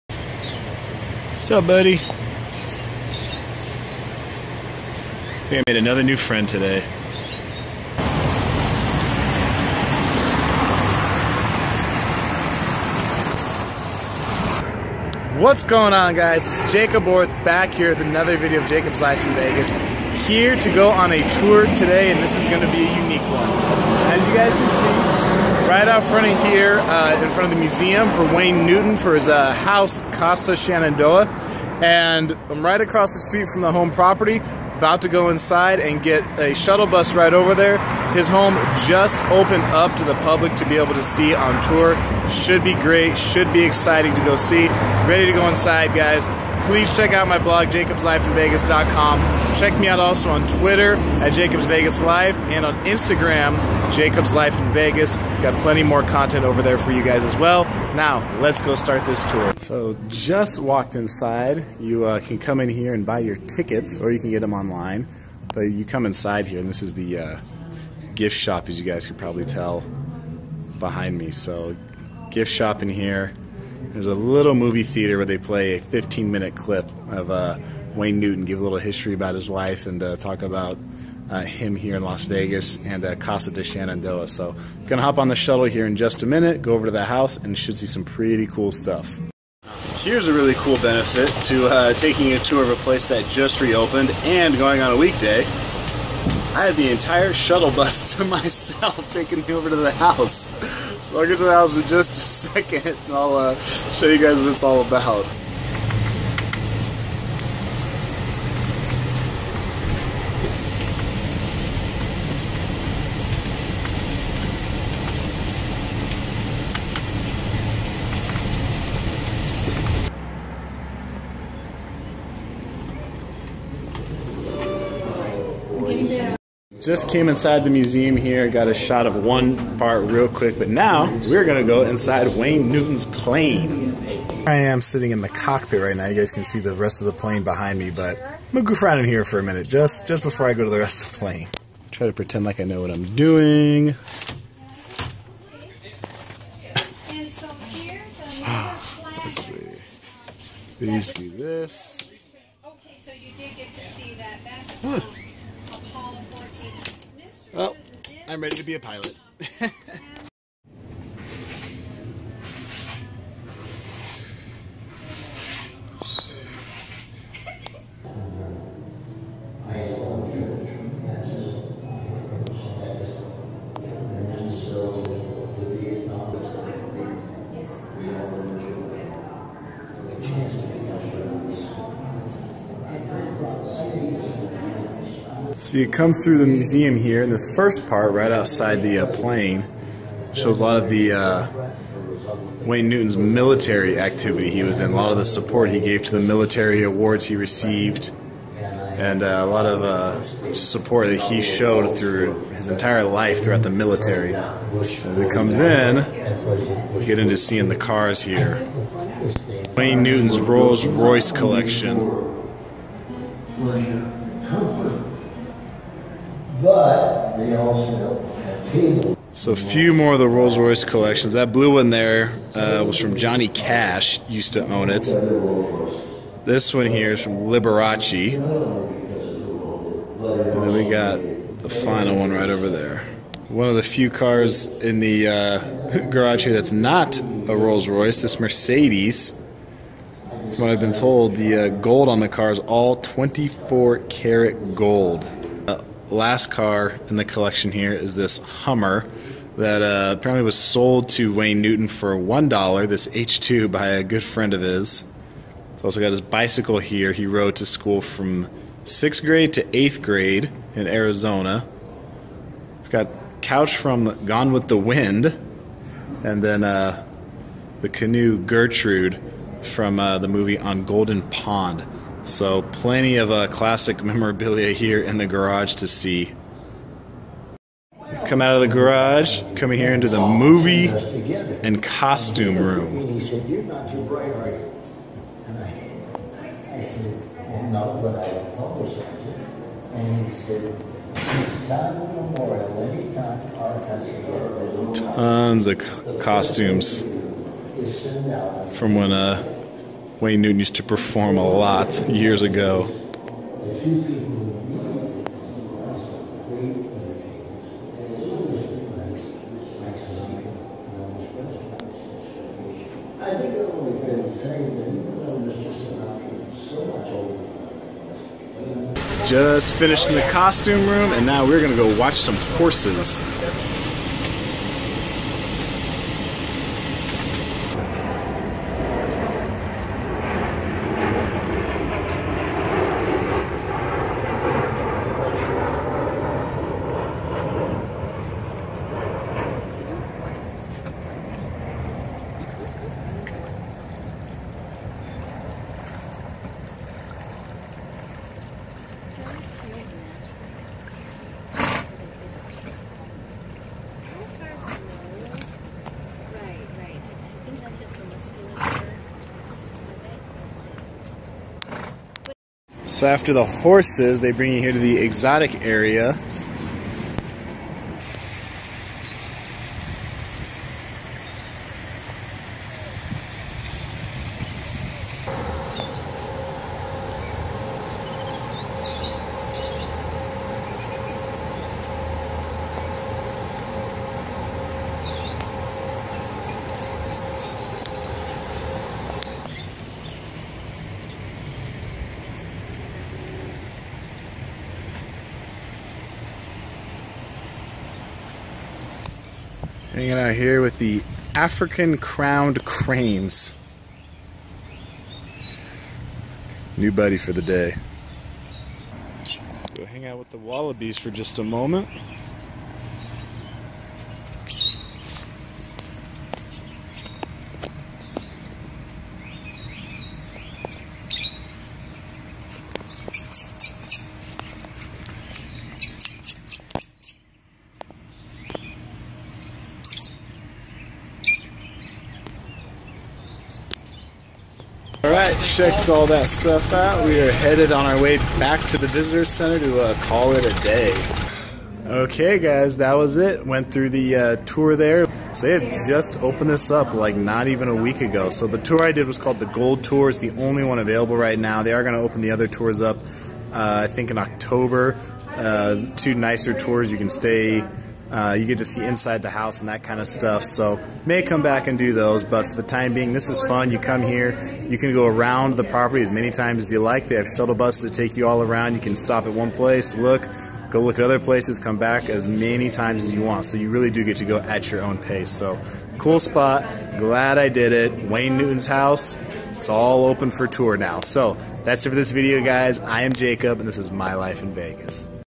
Las Vegas Tour: Wayne Newton Home Casa de Shenandoah
I jumped on this Las Vegas tour as soon as I heard it was opening! Wayne Newton’s famous house “Casa de Shenandoah” was just reopened for public tours in September, 2015. Wayne Newton has a long history here in Las Vegas that eventually earned him the name “Mr. Las Vegas.”

  I went on a weekday to do this Las Vegas tour because I felt that would be when it was least crowded. I was correct that it was not very crowded. Also, a lot of people are not aware yet that the house is even available to do as a Las Vegas tour.

  The home is located at 3310 East Sunset Road in Las Vegas at the intersection of Pecos Road on the east side of Las Vegas. The tour I did was called the “Gold Tour.” It was the only one available at the time, and the cost was $35. This Las Vegas tour was well worth the money! You start off at the gift shop across the street from the property. There you watch a 15 minute video that gives a brief history of Wayne Newton’s life and influence in Las Vegas. From there you take a shuttle bus over to Casa de Shenandoah. The 62 acres is absolutely beautiful to see when you are driving in.

  The shuttle drops you off at the museum. Inside is where you can see a lot of Wayne Newton’s military memorabilia and awards he received for all of the support he had shown the military over his life. At one end of the military memorabilia is where Wayne Newton’s private terminal is that goes directly into his Fokker F-28 twin engine private jet! In the jet you are allowed to sit in the cockpit, have your photo taken, and walk throughout the the jet.

  After you see the jet, and then pass through the military memorabilia, you can go into the garage and see Wayne Newton’s car collection. This was one of the cooler parts of this Las Vegas tour! Newton has 12 cars parked in his collection in the garage-7 of them are Rolls Royces!

  Connected to the far end of the garage is Newton’s costume room. This is a smaller room, but it has a very nice display of costumes that he wore back when you was performing regularly.

  From the costume room the shuttle bus will take you to see Newton’s Arabian horses. I got the chance to see one of the horses do a swimming workout in the pool on his property. During the workout the tour guides did a great job of answering questions and giving people background information about the horses on the property.

  After seeing the horses you get back on the shuttle bus to see the “Exotic Animals” section. Here is where you get to see parrots, cockatiels, peacocks, a black swan, a monkey, wallabies, and African Crowned cranes. This area alone has more than enough interesting life to see, especially for those of you that are nature lovers!

  Since I did the “Gold Tour” from here you can either end your day and go back to the gift shop. However, if you like you can simply take the shuttle bus back to another stop that you were at before. There is no time limit set on how long you can be on the tour. Just understand that with the Gold Tour that you do not have the ability to actually go inside of the home.

  At the end of it I would say that visiting Wayne Newton’s Home Casa de Shenandoah was a great experience! This Las Vegas tour is family friendly, appropriate for all ages, and certainly for those that are big time Wayne Newton fans. If you really want to see into the life of Wayne Newton and get to see some of “Old Vegas” then this is the Las Vegas tour for you!